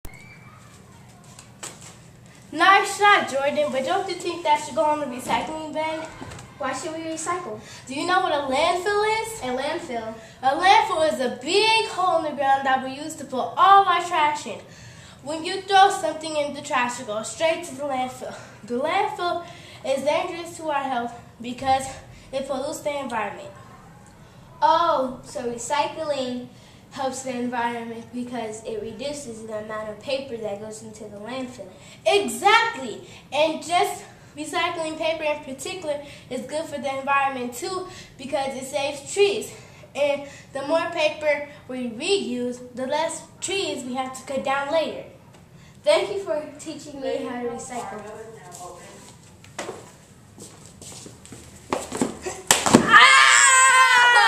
Recycling PSA
The girls made this PSA as part of GREEN:  It's the new pink!